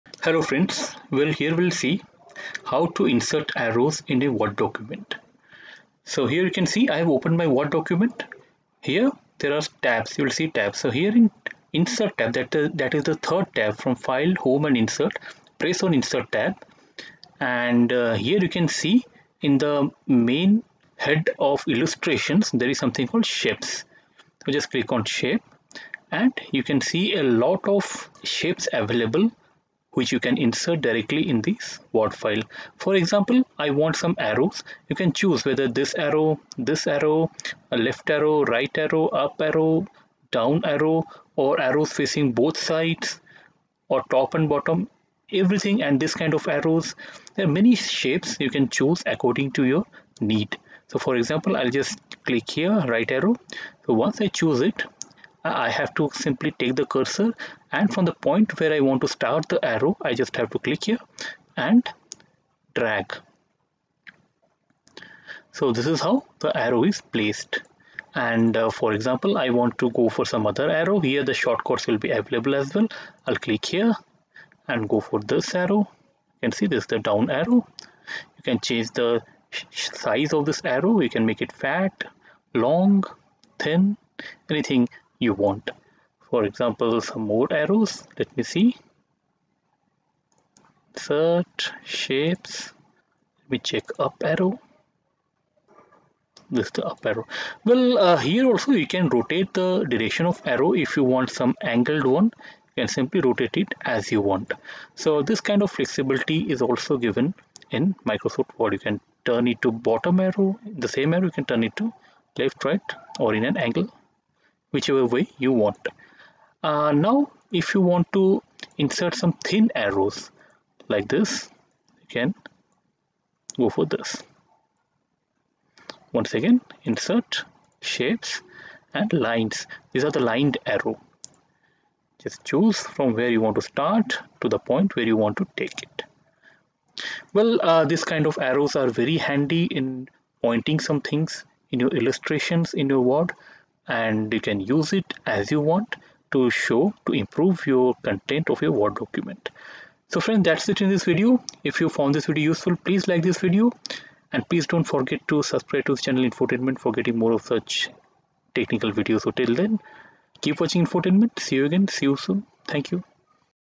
How to Insert Arrows in Word File
How to insert arrow in word: how to make an arrow in Microsoft word.

How to Insert Arrows in Word
Open the Word document that you want to insert arrows. ...
There is "Insert" tab at the top of the word document. ...
Hit the "Illustrations" tab and there you can see the "Shapes" to get the work done.
Click "Line and Connectors" option to access the type of shape i.e. arrow.
More items...